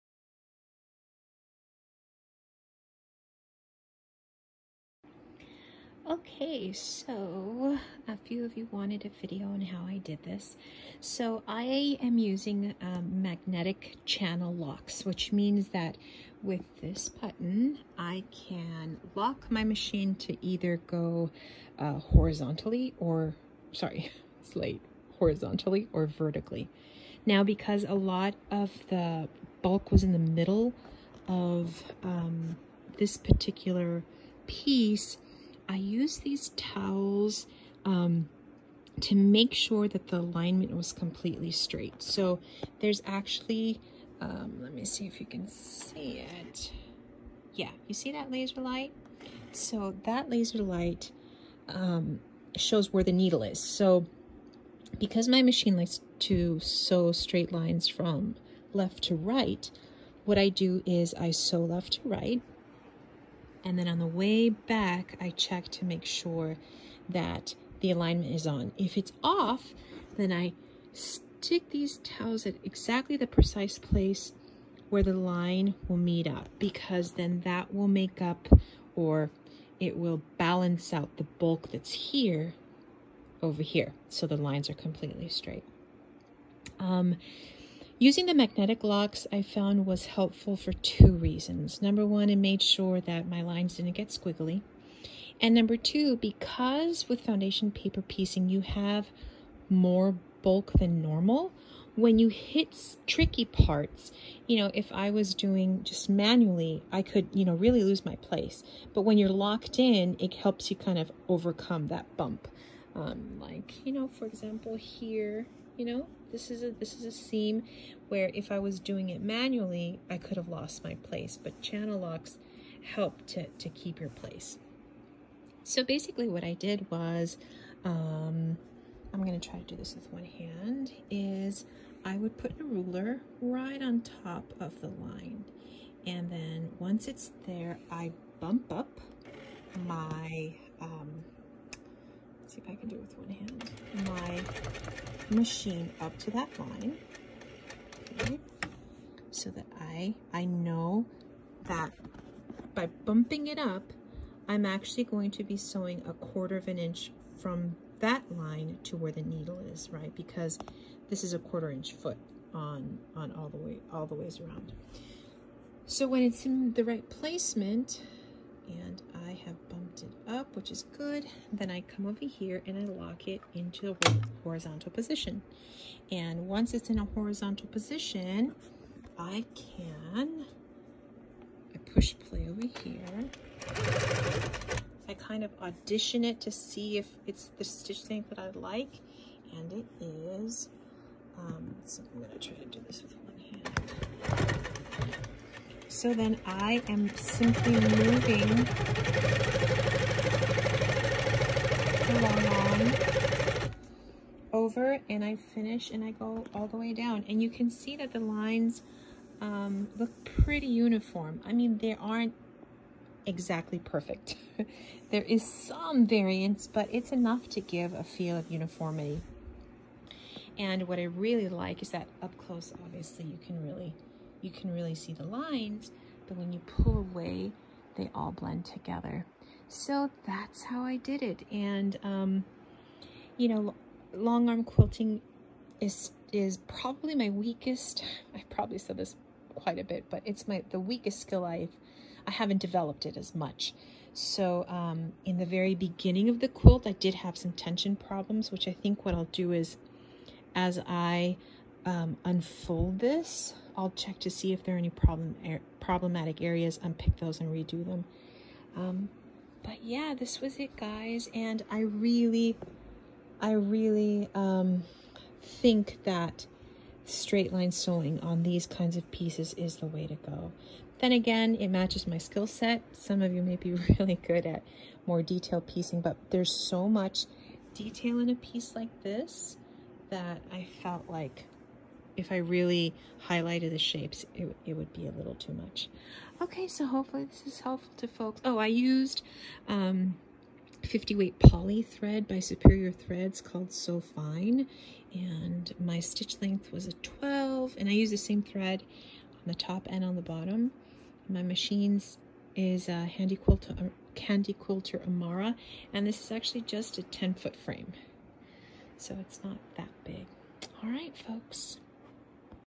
Quilting a Foundation Paperpieced Quilt (Matchstick lines)
Foundation Paper Piecing. This video demonstrates how I quilted 1/4 in horizontal lines on a large scale foundation paper pieced quilt.

This is an original art quilt by veruschka zarate of @prideandjoyquilting6062

Product Information:
Quilt Fabric is by @modafabrics line of Bella Solids
Thread used for piecing is by @WonderFilThreads Decobob 80wt poly color smoke
Thread used for quilting is by @superiorthreads So Fine 50wt poly color 417 Antique Rose
Long Arm machine is @HandiQuilter amara with a 10foot frame with magnetic channel locks.